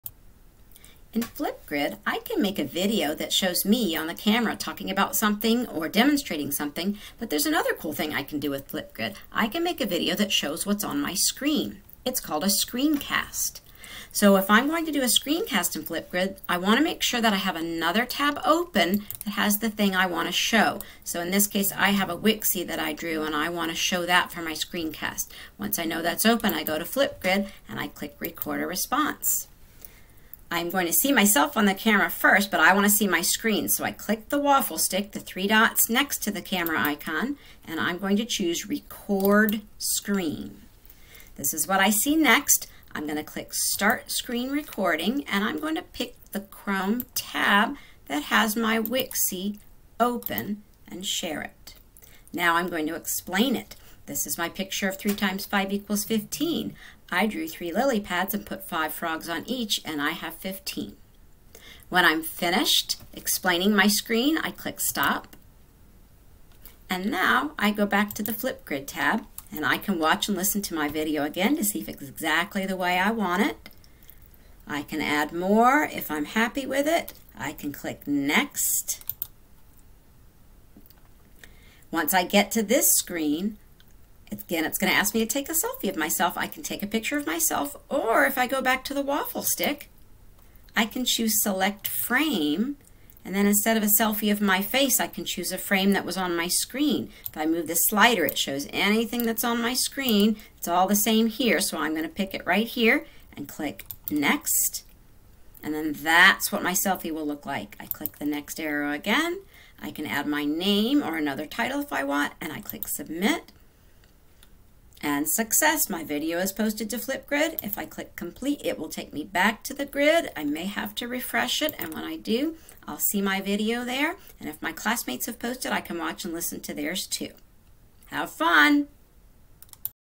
Virtual Learning Tech Tips: Flipgrid Screencast
Our expert SBTS from Deer Park ES is back to show us how to use Flipgrid to make a screencast! It's a fun way for students to share their work with the teacher and classmates. Parental permission will be required for student’s to use this app.